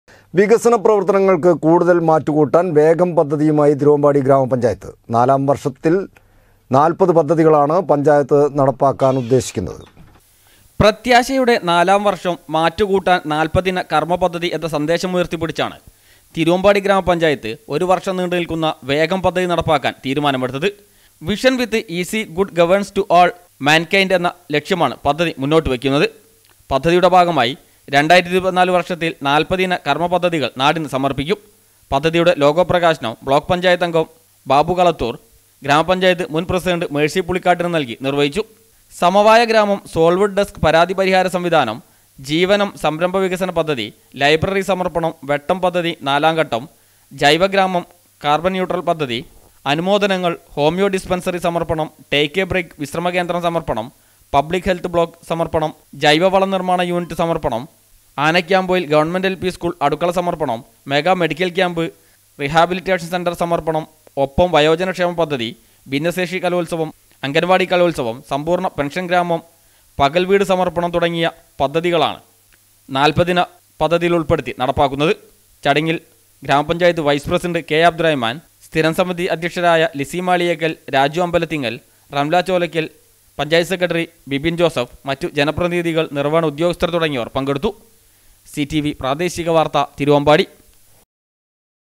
വികസന പ്രവർത്തനങ്ങൾക്ക് കൂടുതൽ മാറ്റുകൂട്ടാൻ വേഗം പദ്ധതിയുമായി തിരുവമ്പാടി ഗ്രാമപഞ്ചായത്ത്
വികസന പ്രവർത്തനങ്ങൾക്ക് കൂടുതൽ മാറ്റുകൂട്ടാൻ വേഗം പദ്ധതിയുമായി തിരുവമ്പാടി ഗ്രാമപഞ്ചായത്ത്.നാലാം വർഷത്തിൽ 40 പദ്ധതികളാണ് പഞ്ചായത്ത് നടപ്പാക്കാനുദ്ദേശിക്കുന്നത്.